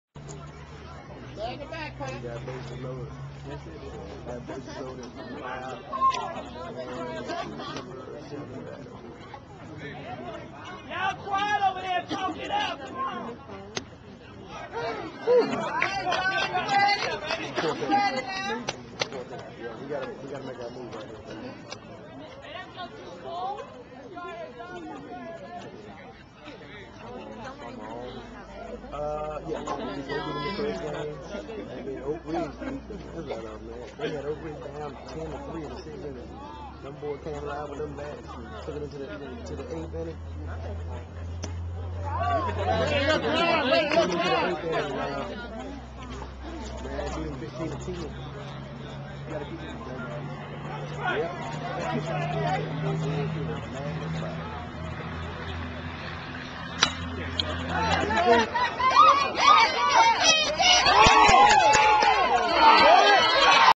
Northern Vance Baseball .John Nelson
#24 IN WHITE , GETS AN INFIELD HIT ON SPEED